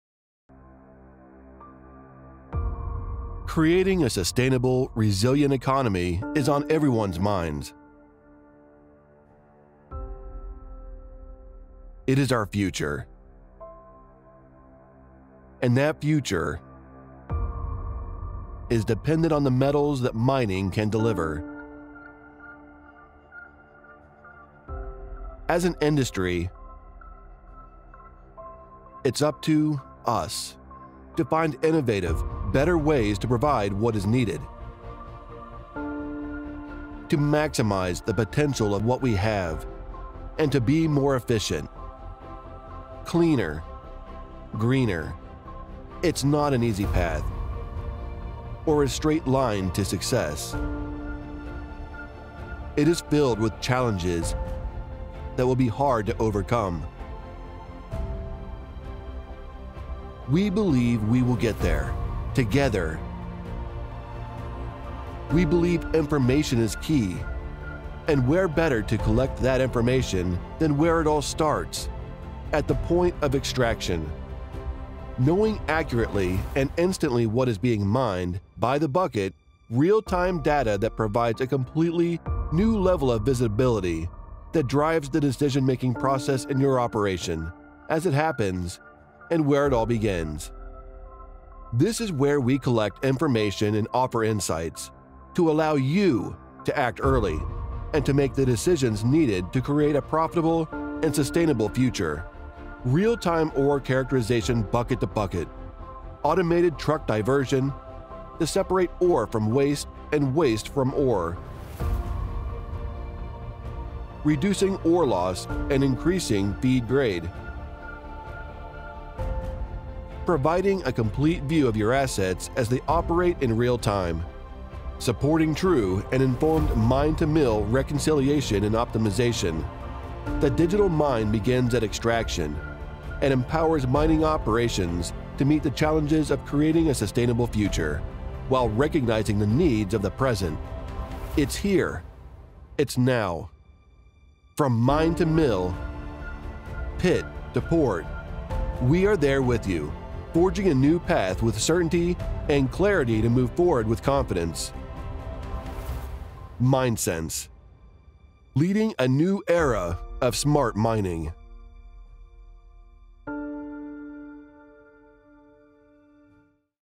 MineSense: Leading a new era in smart mining
MineSense is leading a new era of smart mining with its internationally acclaimed, ShovelSense® smart shovel that characterizes ore in every bucket, enhances orebody knowledge, and enables automated ore sorting at extraction to greatly increase profitability and sustainability.